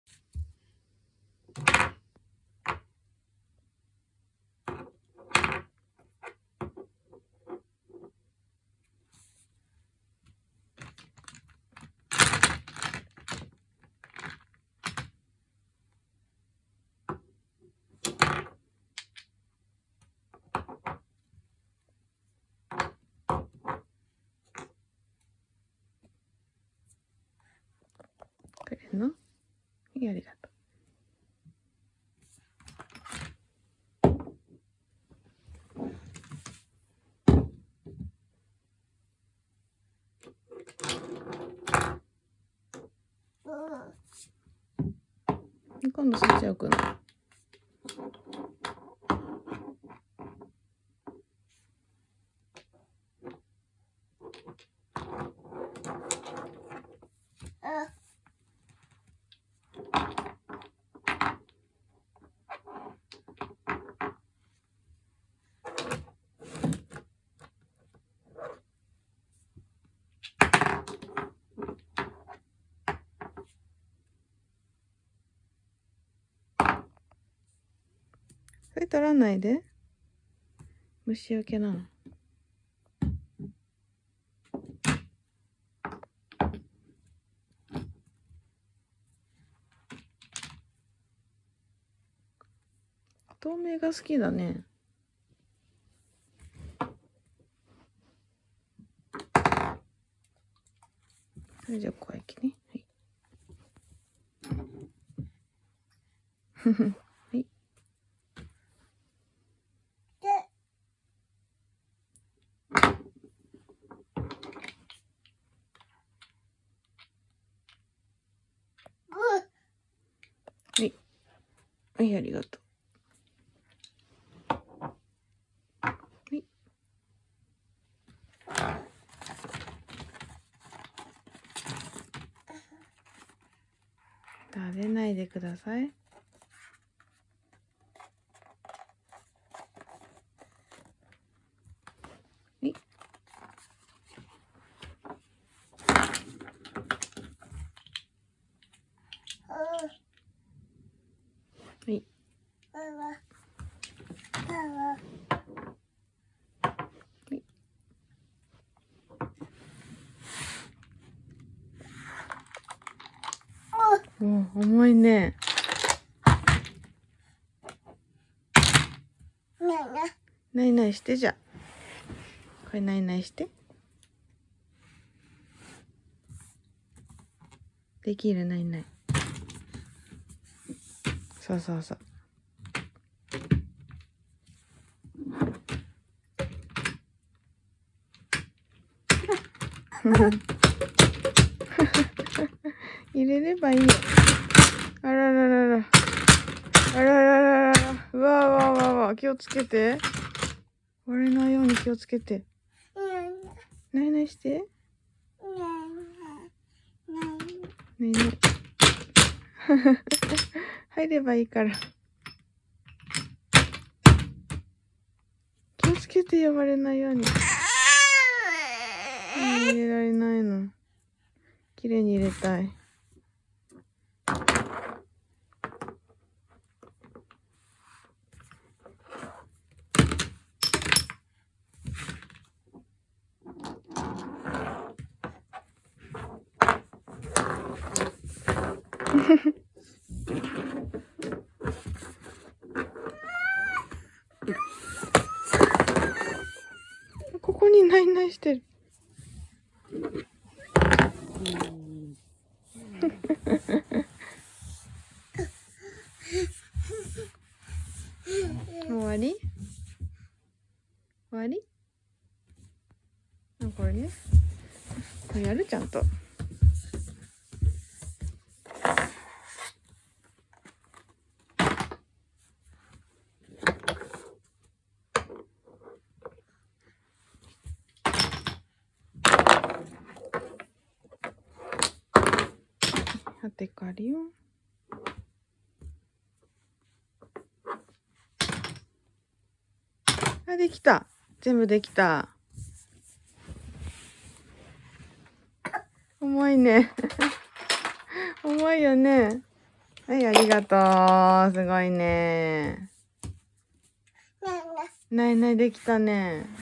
1歳5ヶ月が真剣な顔してマニキュアを物色するだけ/1 year old baby playing with nail polish with serious face
もうマニキュアなんて一生塗らないかもしれないのに捨てられません〜